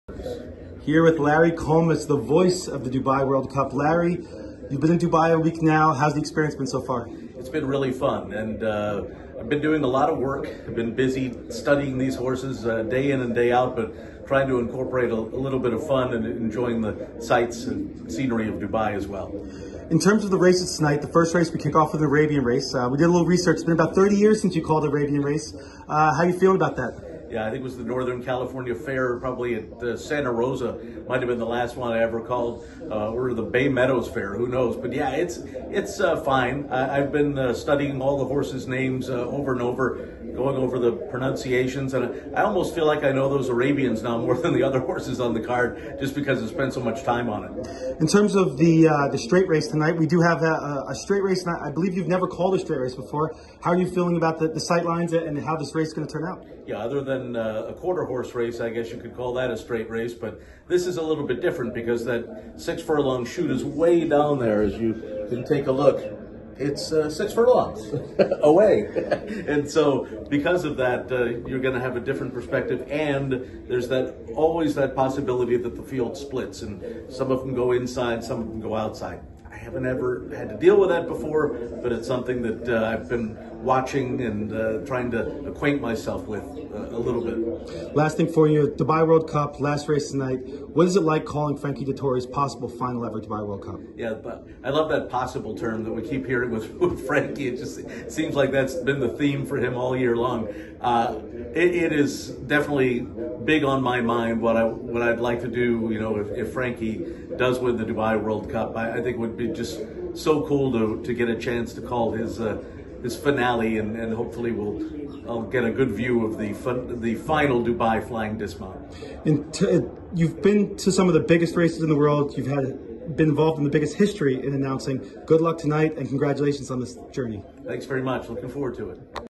Announcer Larry Collmus from Dubai on calling Dubai World Cup night
Andrew talks with announcer Larry Collmus at the start of Dubai World Cup night